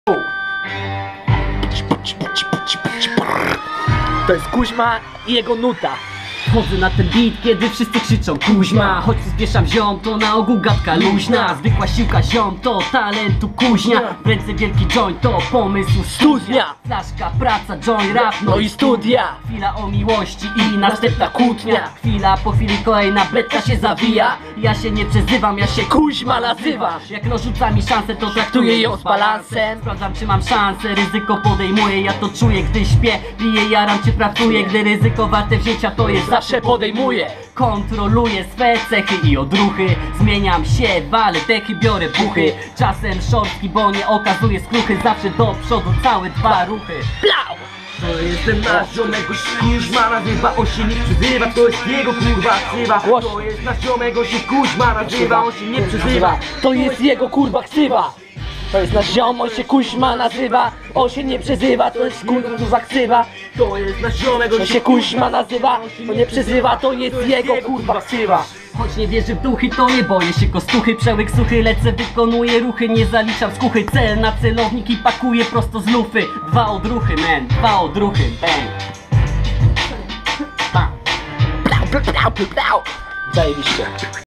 Kuzma Rap Session
Spontant z chlopakami, silka joincik rapsy !

Location: Bournemouth